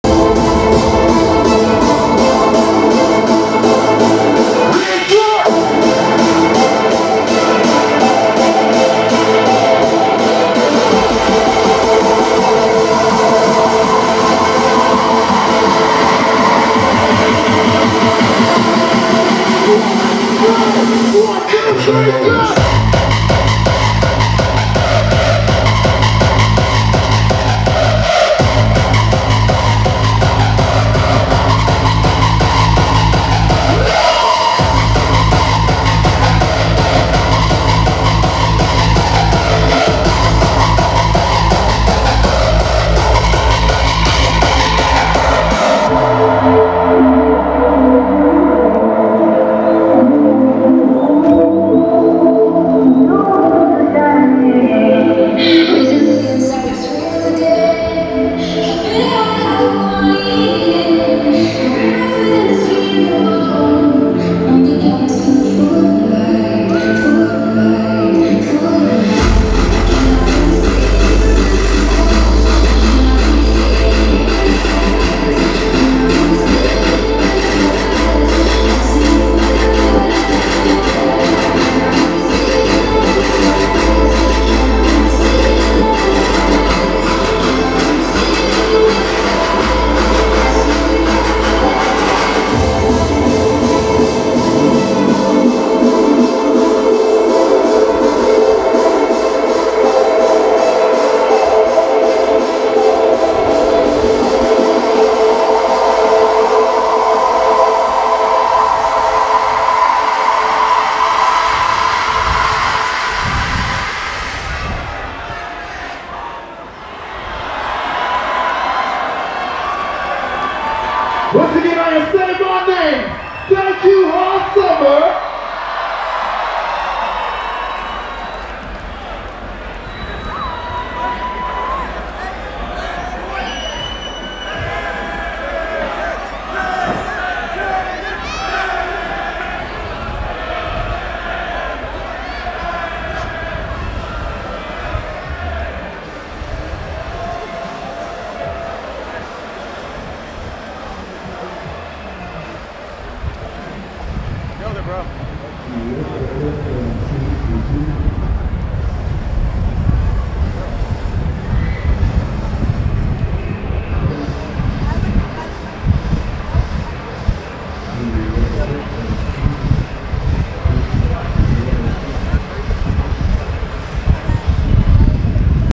SAYMYNAME - THE END OF THE SET @ HARD SUMMER 2018 - 8.4.2018
SO... THE HOMIE MEG ASKED IF I WOULD BE DOWN TO ROLL TO HARD SUMMER 2018... AND I SAID YEAHHHHH BUDDD

IT WAS A SWELTERING 99 DEGREES ON THE SURFACE OF FONTANA BUT THE VIBES WERE GOING STRONG AND I TRIEDDDD TO CATCH AS MUCH AS I COULD FOR THE REAL FANS.  I DON'T EVEN MONETIZE THIS SHIT SO THESE CLIPS ARE FOR THE REAL ONES YOU HEARDDDDD?